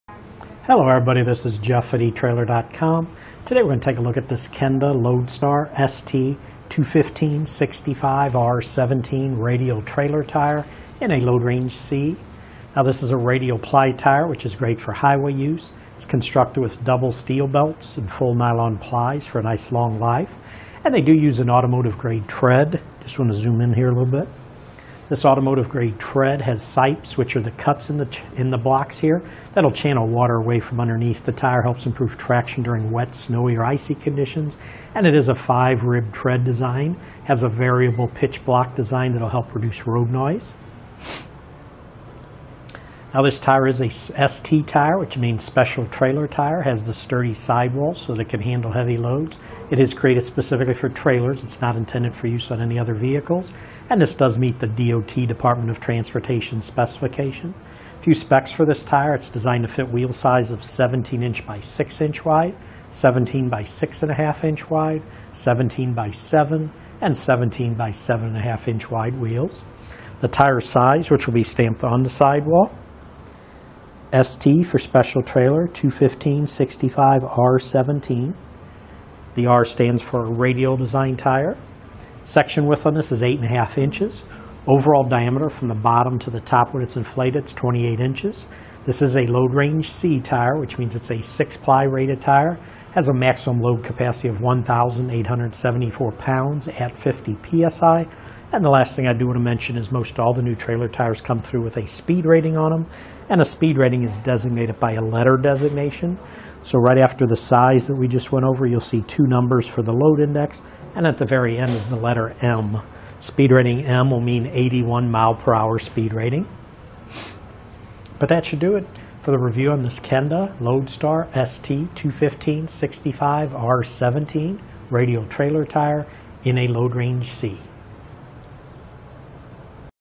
etrailer | Ultimate Overview of the Kenda Loadstar ST215/65R17 Radial Trailer Tire
Click for more info and reviews of this Kenda Trailer Tires and Wheels:

Jeff:                                                  Hello everybody, this is Jeff at etrailer.com. Today we're going to take a look at this Kenda Loadstar ST215/65R17 radial trailer tire in a Load Range C. Now this is a radial ply tire, which is great for highway use. It's constructed with double steel belts and full nylon plies for a nice long life, and they do use an automotive grade tread. Just want to zoom in here a little bit. This automotive grade tread has sipes, which are the cuts in the blocks here.

That'll channel water away from underneath the tire, helps improve traction during wet snowy or icy conditions, and it is a five rib tread design. It has a variable pitch block design that will help reduce road noise.Now this tire is a ST tire, which means Special Trailer tire. It has the sturdy sidewalls so that it can handle heavy loads. It is created specifically for trailers, it's not intended for use on any other vehicles, and this does meet the DOT Department of Transportation specifications. A few specs for this tire.

It's designed to fit wheel size of 17 inch by 6 inch wide, 17 by 6 1/2 inch wide, 17 by 7, and 17 by 7 1/2 inch wide wheels.The tire size, which will be stamped on the sidewall ST for Special Trailer 215/65R17. The R stands for Radial design tire. Section width on this is 8 1/2 inches, overall diameter from the bottom to the top when it's inflated is 28 inches. This is a Load Range C tire, which means it's a six ply rated tire. It has a maximum load capacity of 1,874 pounds at 50 PSI, and the last thing I do want to mention is most all the new trailer tires come through with a speed rating on them and a speed rating is designated by a letter designation.

So right after the size that we just went over, you'll see two numbers for the load index, and at the very end is the letter M. Speed rating M will mean 81 mile per hour speed rating.But that should do it for the review on this Kenda Loadstar ST215/65R17 Radial trailer tire in a Load Range C.